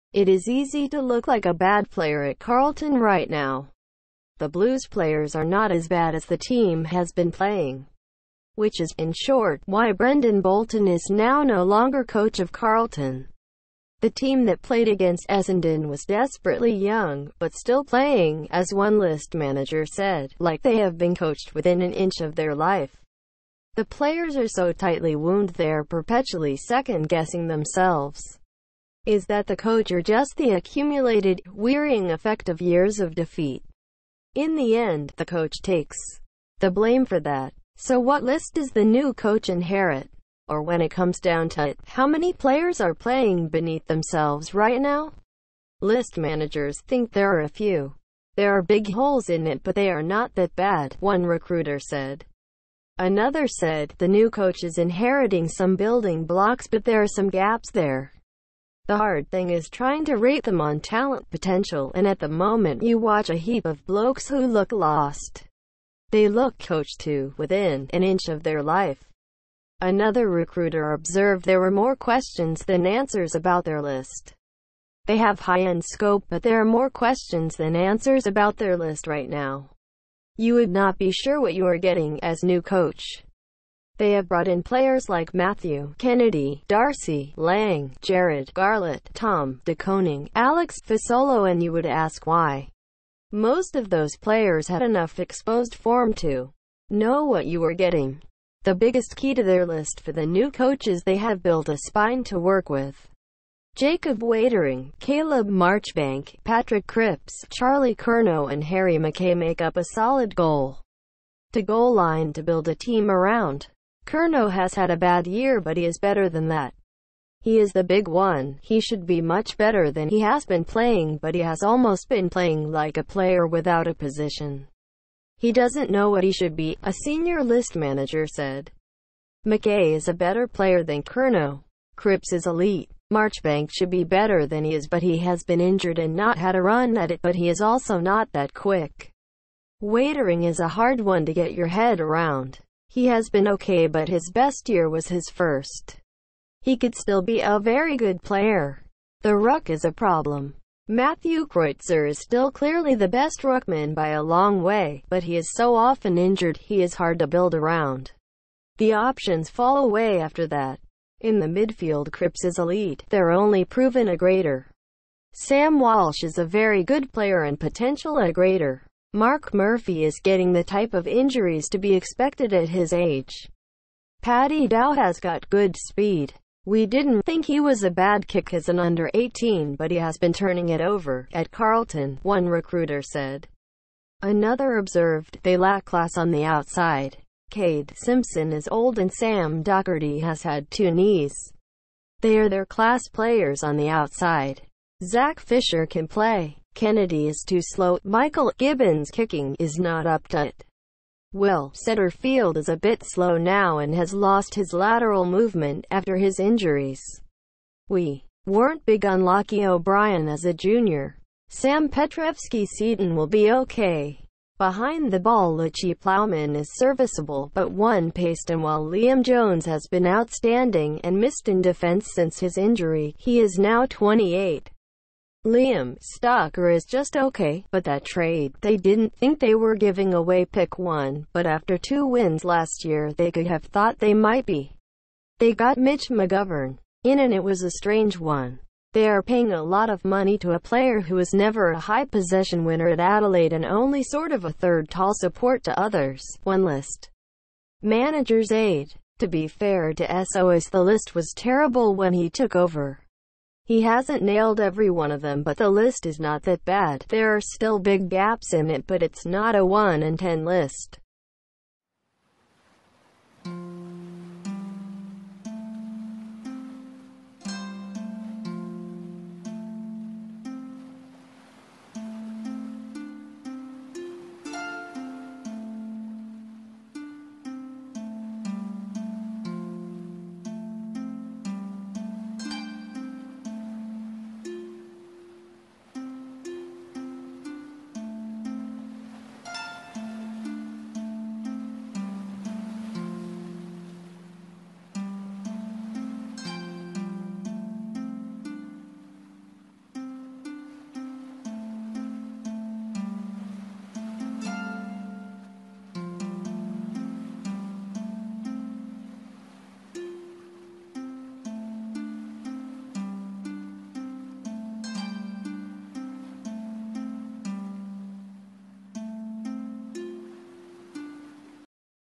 What will the next Carlton coach inherit?
How many players are playing beneath themselves right now? List managers think there are a few.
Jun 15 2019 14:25